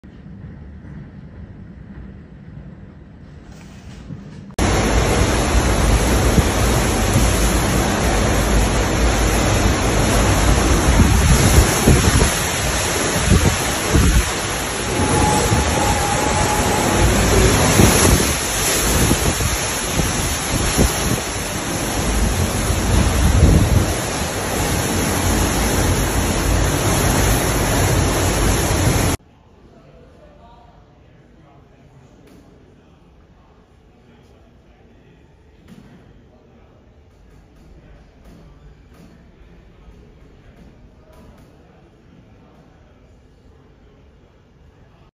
Florida, l’uragano Milton “strappa” via il tetto dello stadio di Tampa
L'uragano Milton ha letteralmente strappato via il tetto dello stadio di Tampa, in Florida. La struttura che ospita la squadra dei Tampa Bay Ray è stata colpita da venti a oltre 100 km/h. Sono oltre 2 milioni le persone rimaste senza energia elettrica per il passaggio dell'uragano Milton.